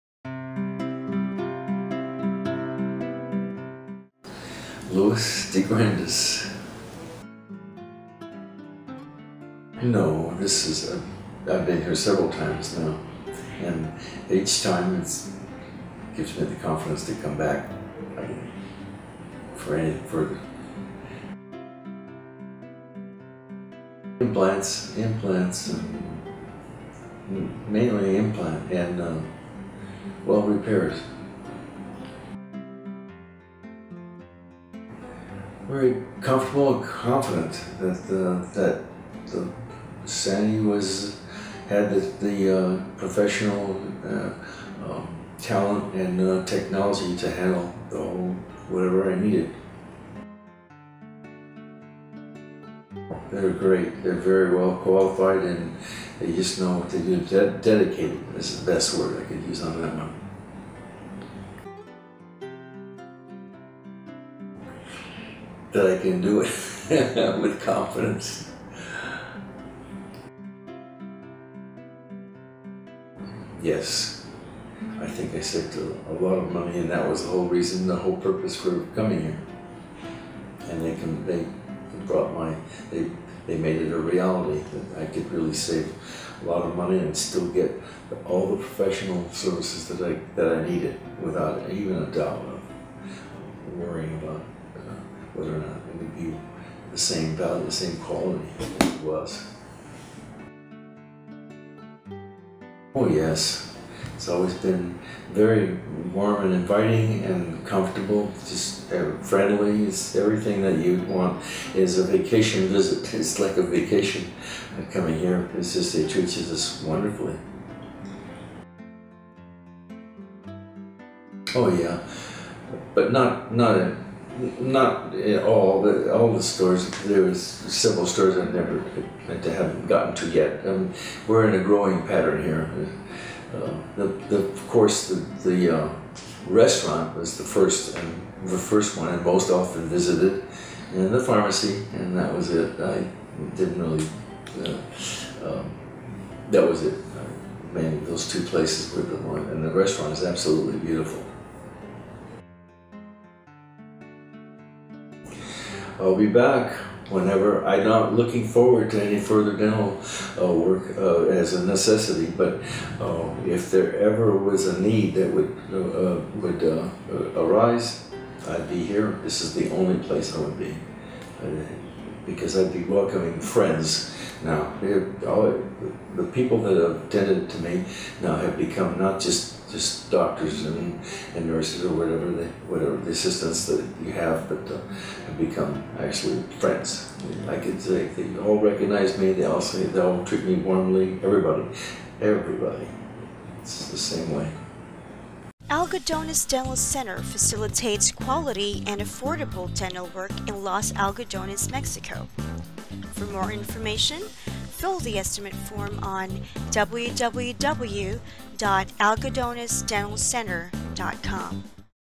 Dental Implant Crown Procedure in Algodones, Mexico | Repeat Patient's Review
"It is like a vacation coming here. It is just wonderful." That's what Louis said when he got dental implants in Los Algodones, Mexico!

0:07 Introduction
0:21 Dental implant procedure in Los Algodones, Mexico
0:50 Staff & dentists in Los Algodones, Mexico
1:11 Cost savings in Los Algodones, Mexico
1:41 Dental tourism in Los Algodones | Patient's opinion on vacation, stores, restaurants & more
2:32 Dental work experience in Los Algodones

In this video, Louis expresses his utmost satisfaction with his treatment, attributing it to the expertise and dedication of the dentists and staff.

He not only gained a newfound confidence in his smile but also experienced significant savings compared to dental treatments in his home country.

Want to know more about the treatment Louis got? Learn about them here!